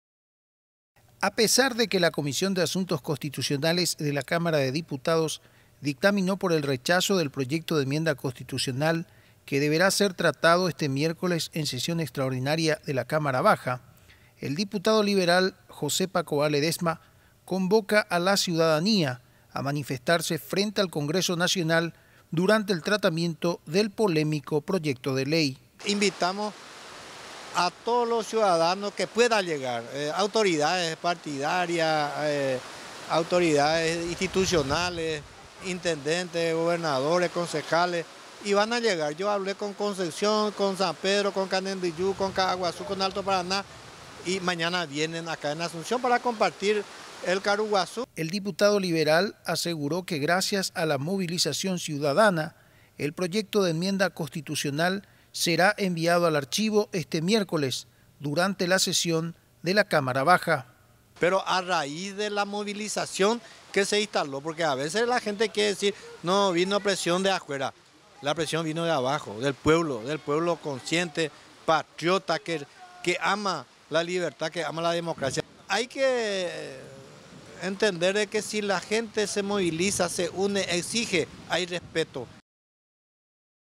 Convocan a la ciudadanía frente al Congreso
Hoy se estaría enviando al archivo el proyecto de enmienda constitucional durante la sesión de la cámara de diputados; no obstante, el diputado José Pakova Ledesma invita a la ciudadanía a congregarse frente al Congreso Nacional.